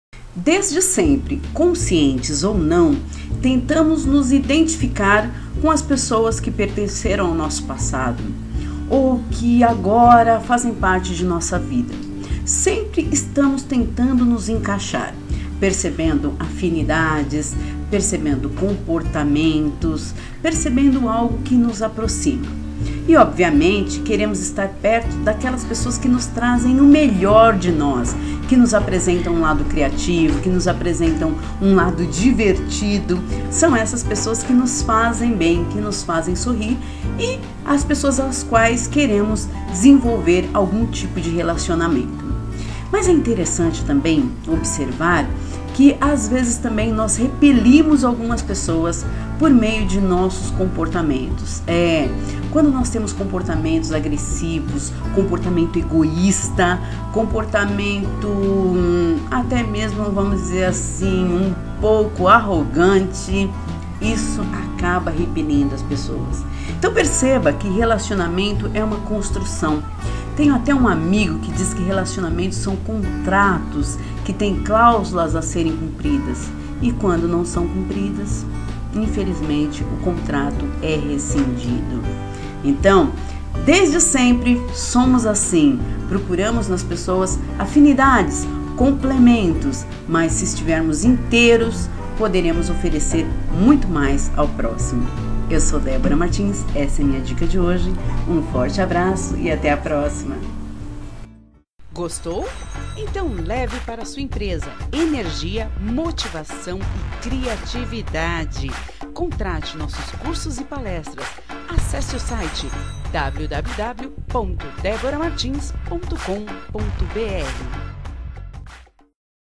Relacionamentos são Contratos
Débora Martins fala sobre a importância dos relacionamentos saudáveis, sustentáveis, baseados no respeito e reciprocidade. "Desde sempre queremos nos relacionar, participar, pertencer. Buscamos por complemento e afinidade." complementa "Toda magia dos relacionamentos reside em nosso comportamento."